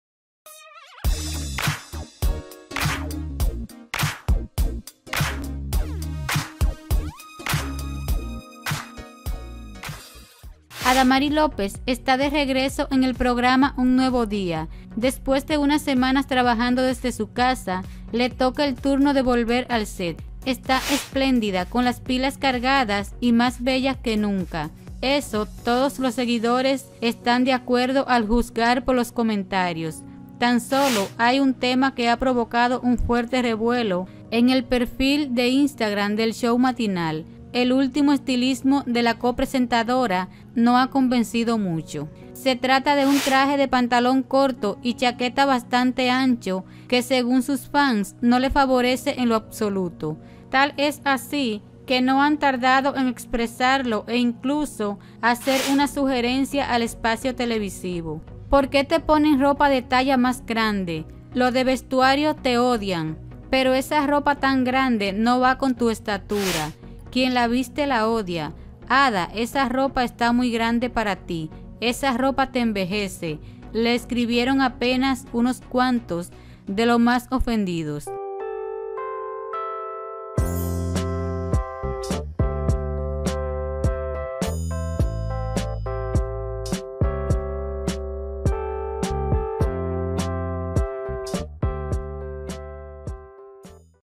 El último estilismo de Adamari López en 'Un nuevo día' genera fuerte controversia.
La conductora elegía un modelo algo diferente a lo habitual que sorprendió a sus seguidores. Las opiniones al respecto fueron para todos los gustos.